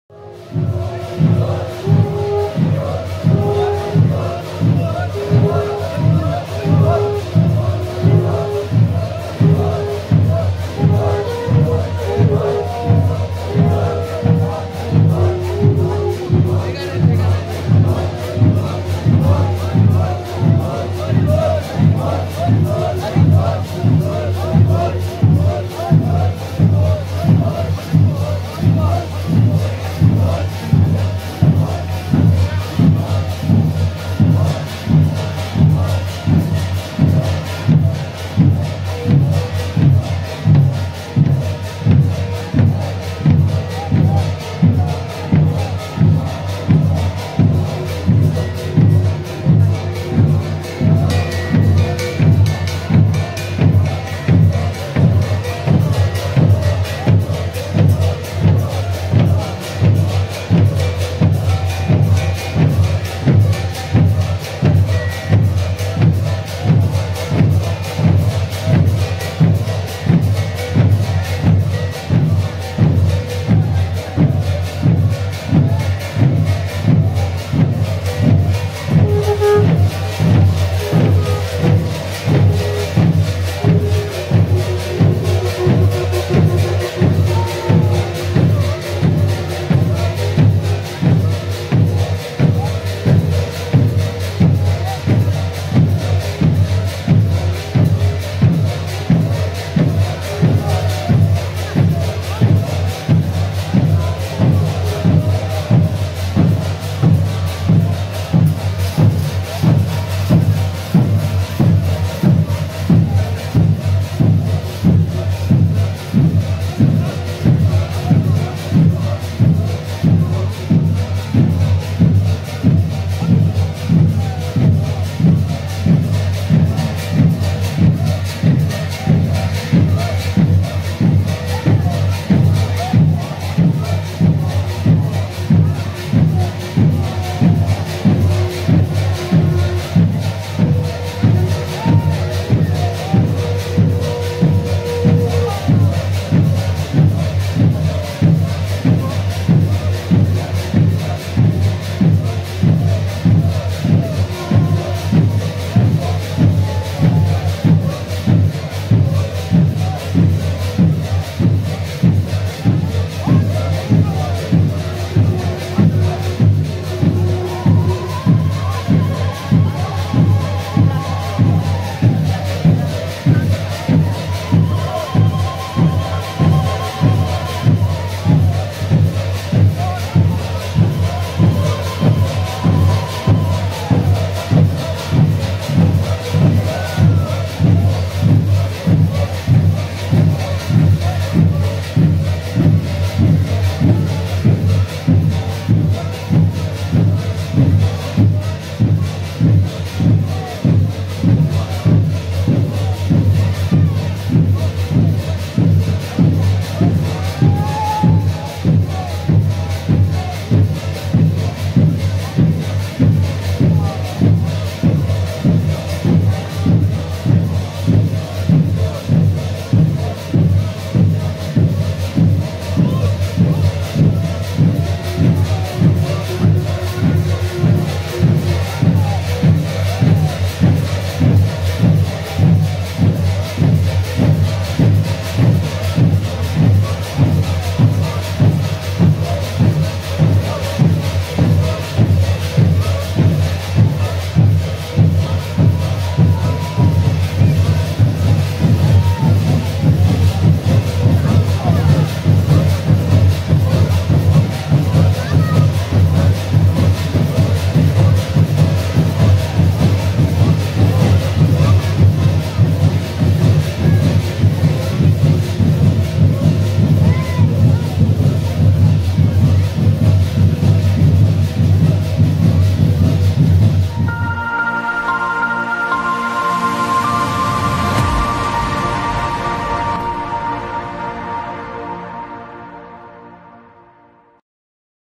Jotadhor Gosai matua team plays extraordinary harinam kirtan at Sridham Orakandi || JAGO MATUA
Channel: Jago Matua 🚩🚩
Admin & Editing: Akash Bala
Also like this video and stay with us for more exclusive video.